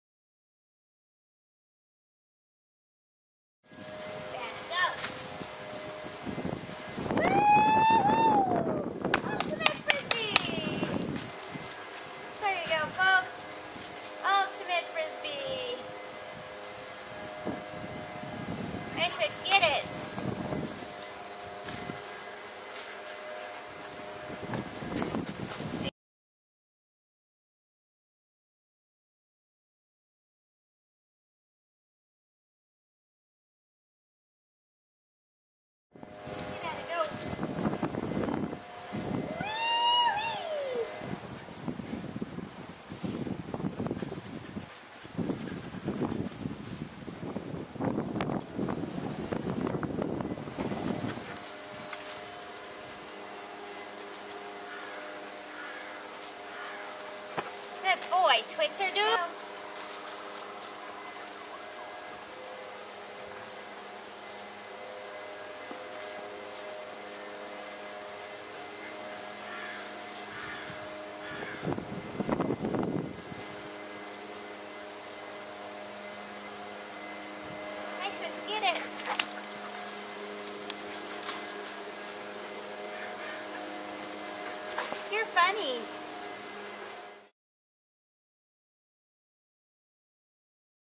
Ultimate Frisbee - Fun dog Games
This is just a fun game that I invented.  :)  Really, all I did was toss a frisbee on my hillside.  LOL!  The funny part is that Bandit will never bring it all the way to me.  He will wait for Twix to go and get it and for Twix to bring it to me.  They are so funny.  They both know their jobs for this game and they stick to it.  Twix will also wait until Bandit drops the frisbee.  They are both really fun dogs and I enjoy spending time with them.  Isabelle wants NO part of this game.  :)  I am sure she was sleeping on my bed in the house.  :)

Pam, Isabelle, Bandit and Twix.